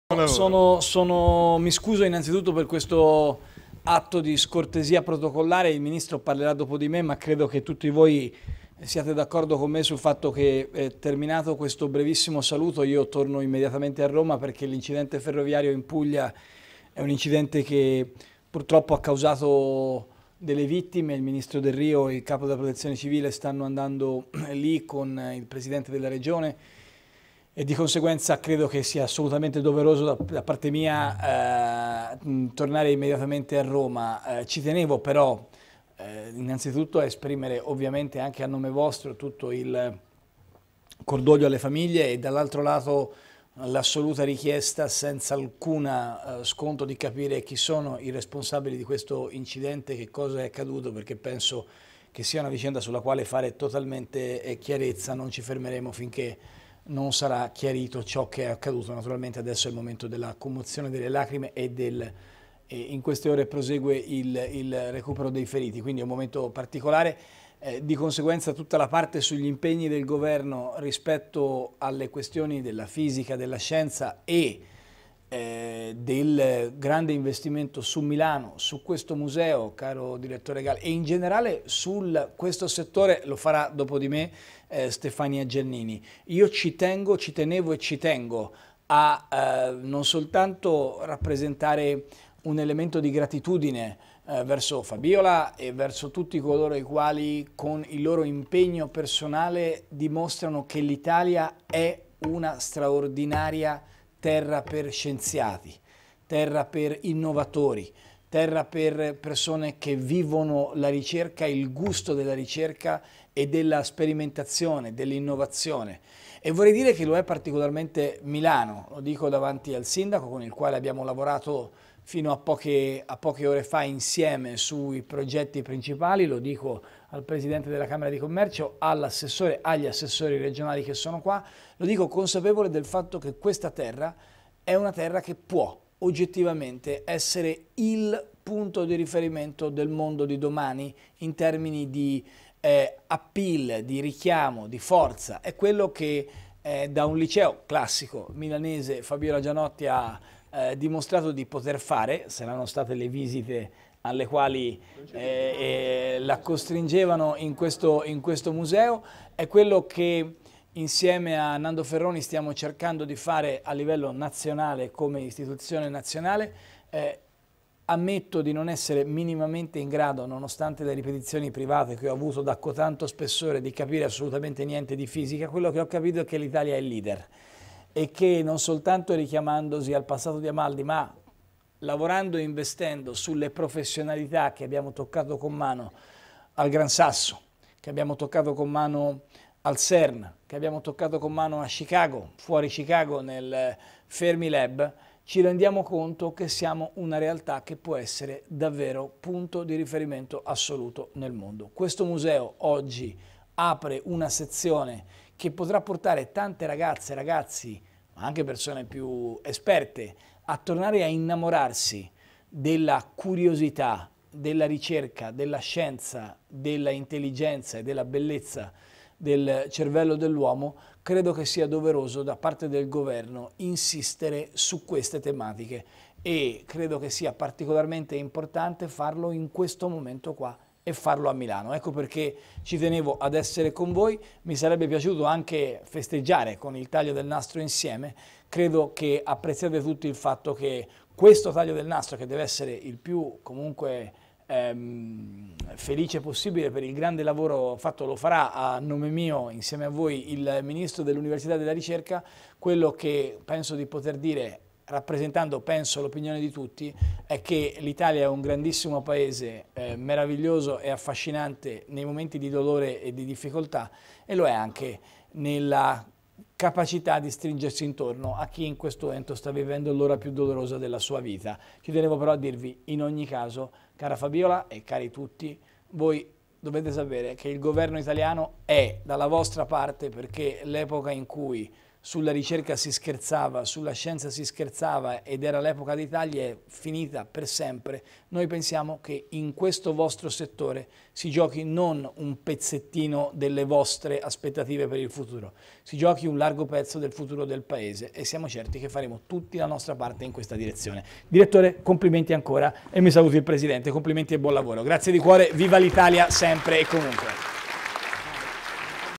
Intervento al Museo nazionale della Scienza e della tecnologia (12/07/2016)
Milano, 12/07/2016 - Il presidente del Consiglio Matteo Renzi è intervenuto all'inaugurazione della mostra “Extreme” presso il Museo nazionale della Scienza e della tecnologia “Leonardo da Vinci”.